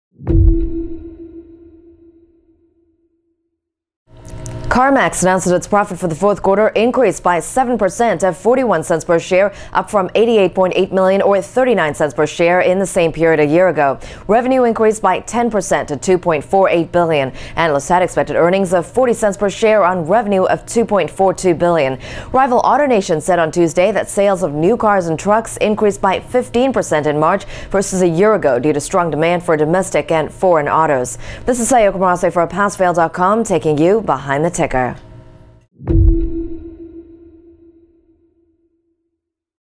Passfail.com News: CarMax Announces Earnings
CarMax (KMX) announced that its profit in the fourth quarter increased by 7% to 41 cents per share, up from $88.8 million, or 39 cents per share in the same period a year ago.  Revenue increased by 10% to $2.48 billion.  Analysts had expected earnings of 40 cents per share on revenue of $2.42 billion.

Rival AutoNation (AN) said on Tuesday that sales of new cars and trucks increased by 15% in March versus a year ago due to  strong demand for  domestic and foreign autos.